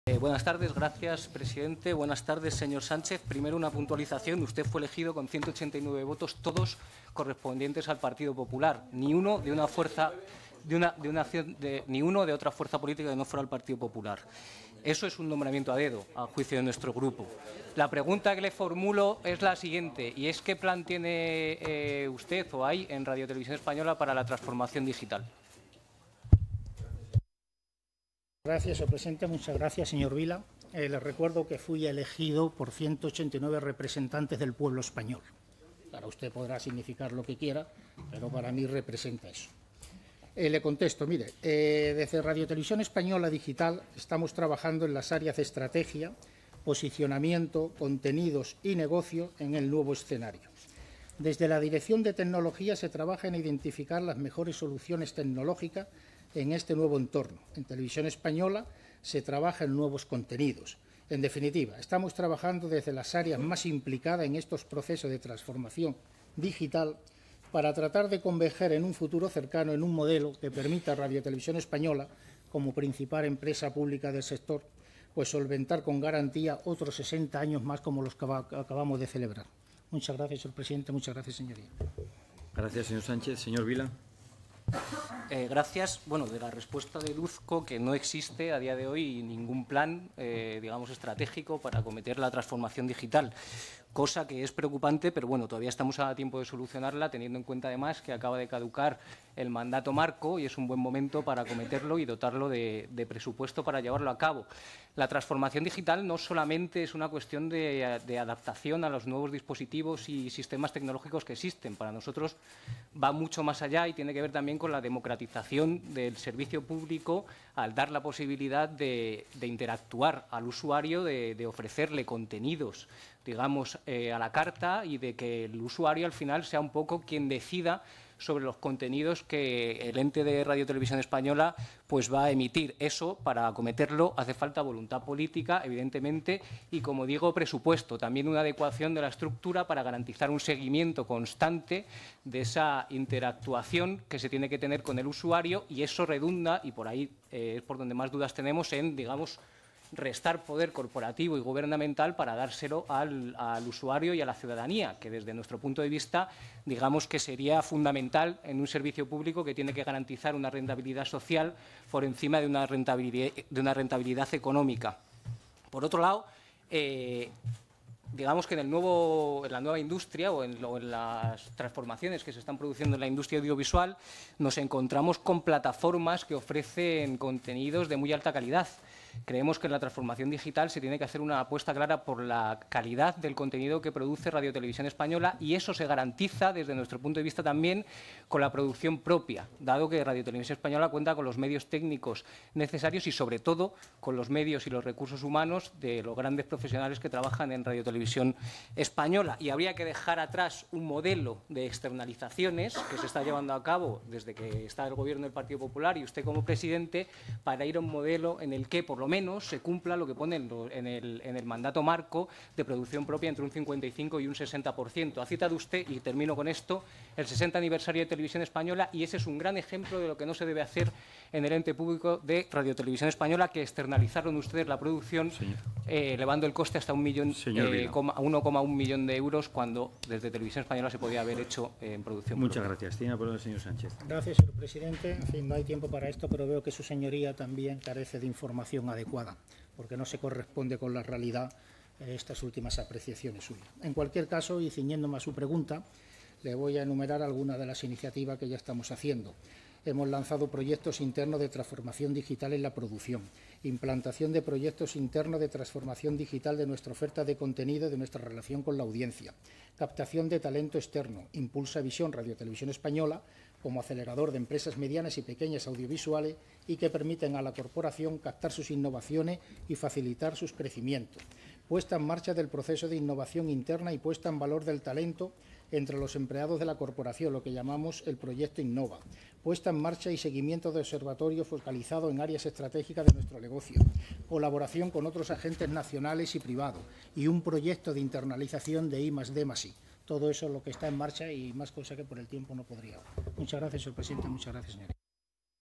Miguel Vila: "¿Qué plan hay para la transformación digital?
Miguel Vila en la Comisión Mixta de Control Parlamentario de la Corporación RTVE y sus Sociedades el 9 de Marzo.